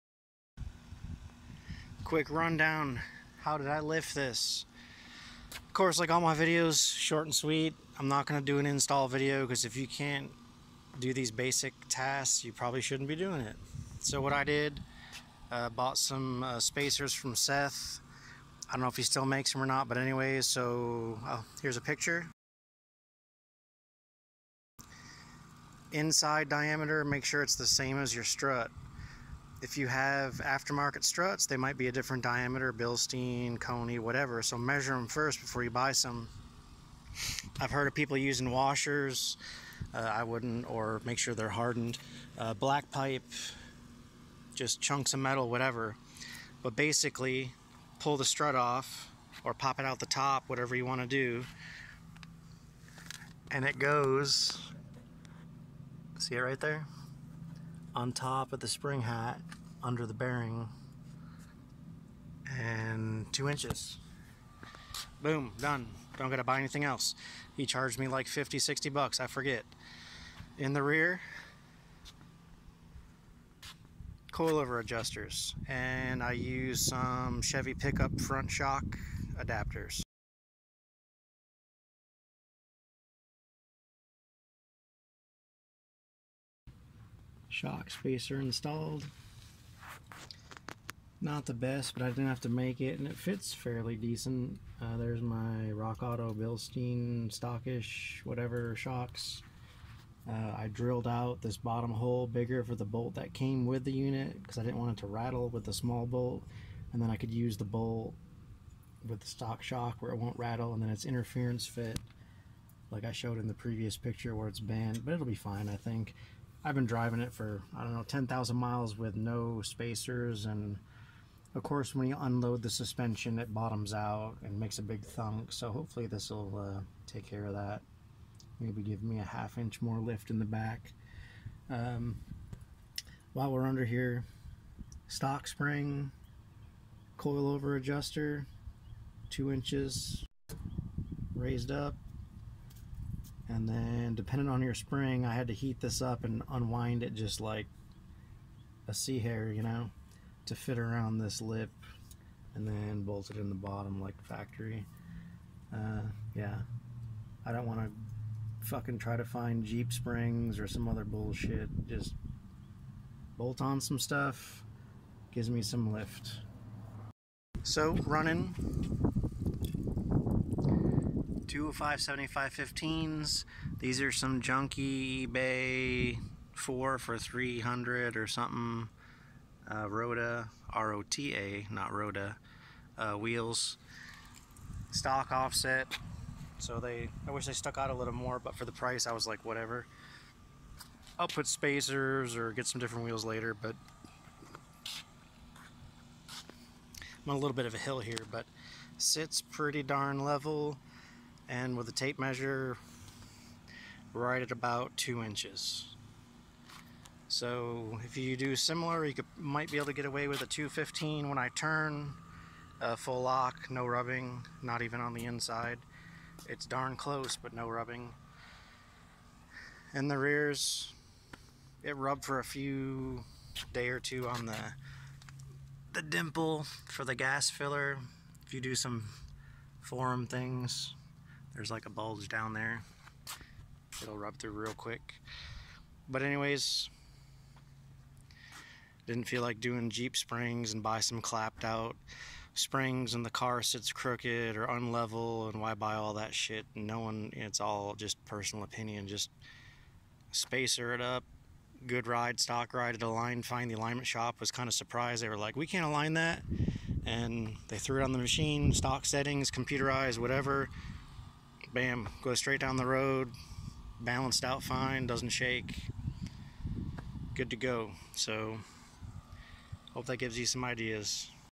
DIY Lift a Mk4 VW
I lifted my 2002 tdi. Fairly easy. Lots of info on the Lifted watercooled VW

Coilover adjusters - i called raceline, they arent on their website and are cheaper than most.
Shock extenders
--Use washers or whatever to space tightly the extenders in your shock pocket, otherwise it will rattle.